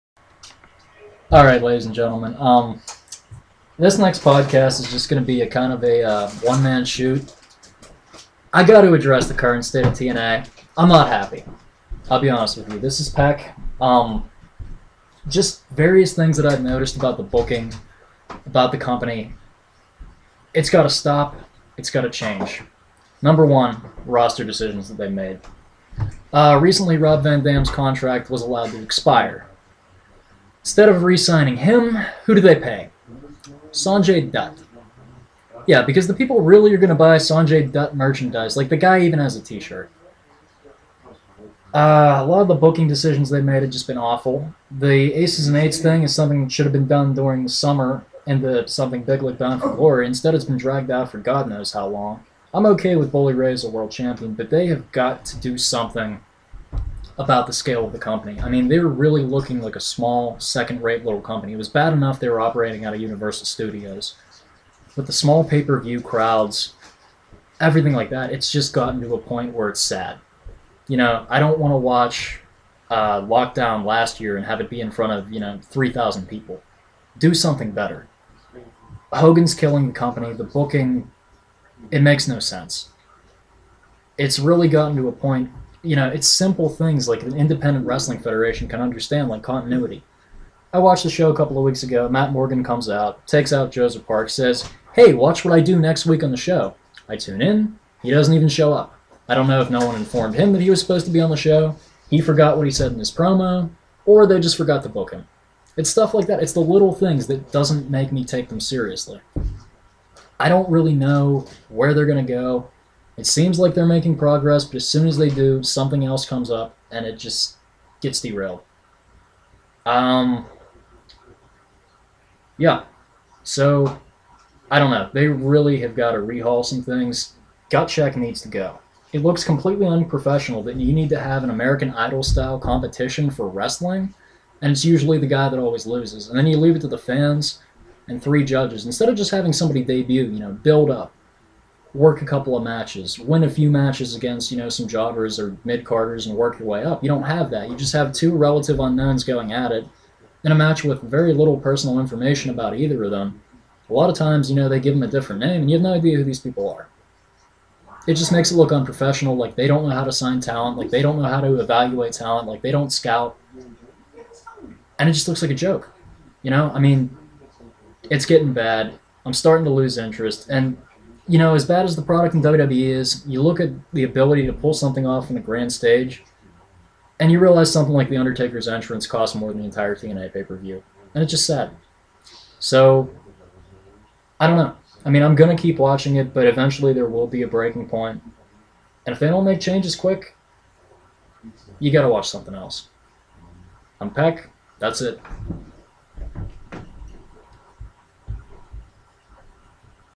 NSR Wrestling Podcast 33: Time for a Rant!
Tyler gives his views about TNA wrestling!